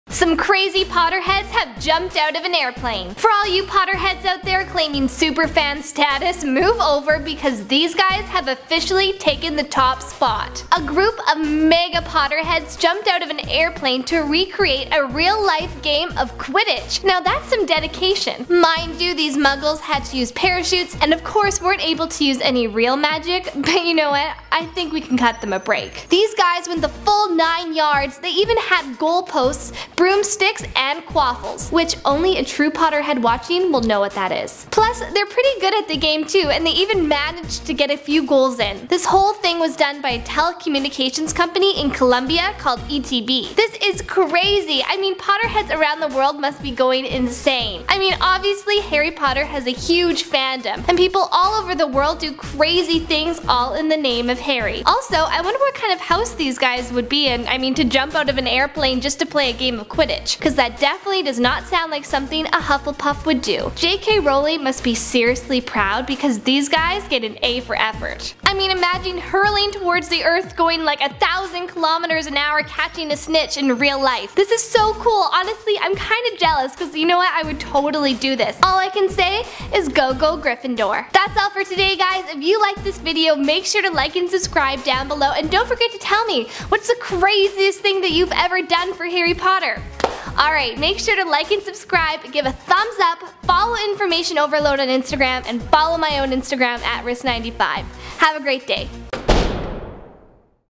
Real Life Quidditch
A group of Crazy Harry Potter fans jumped out of an airplane in order to simulate a game of Quidditch. We've got all the high flying details here for you on iO!

2408 Lakeshore Blvd W.